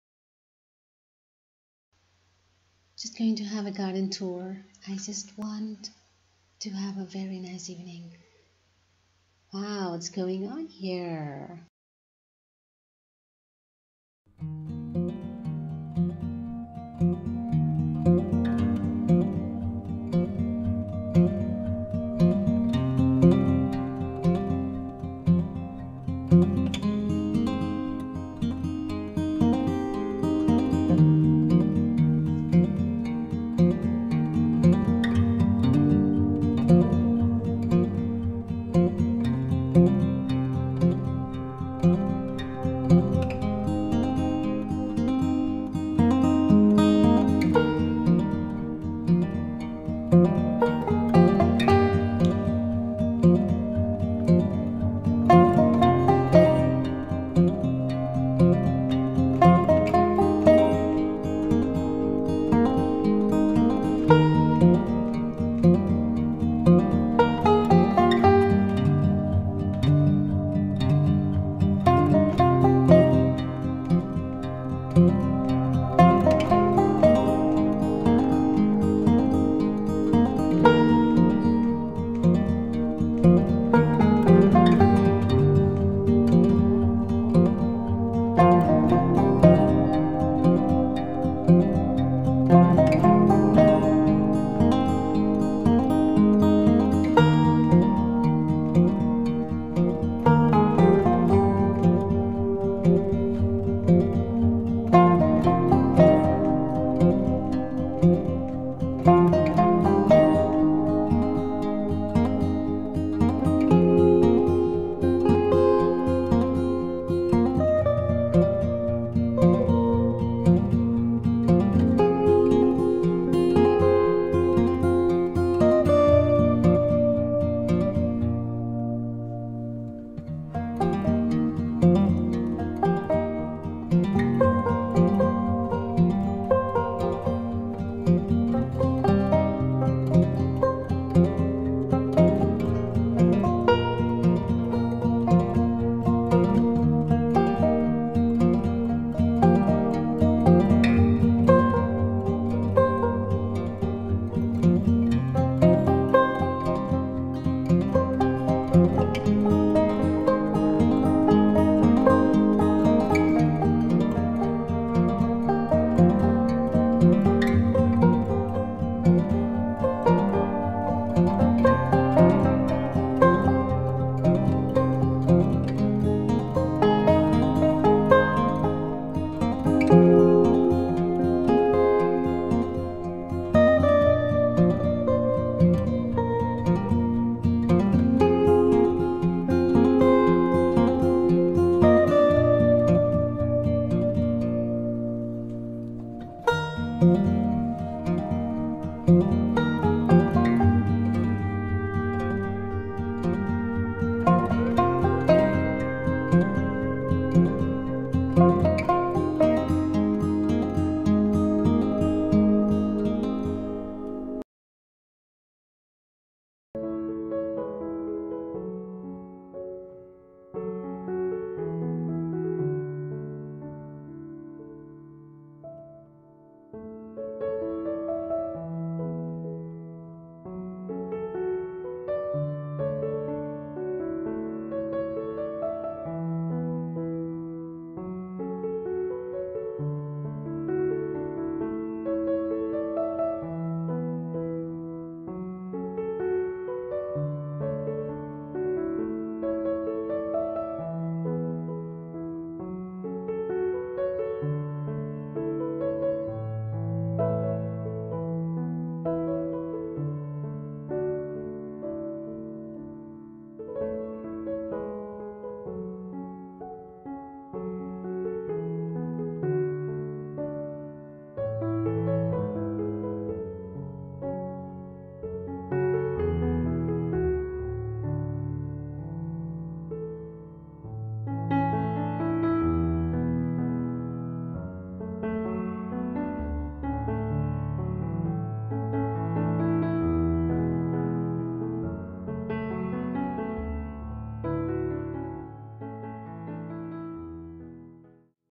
A Blissful Home Garden Tour | Silent Vlog #1
a blissful home garden tour. My first silent vlog. Living a slow hygge life in a small village of Assam.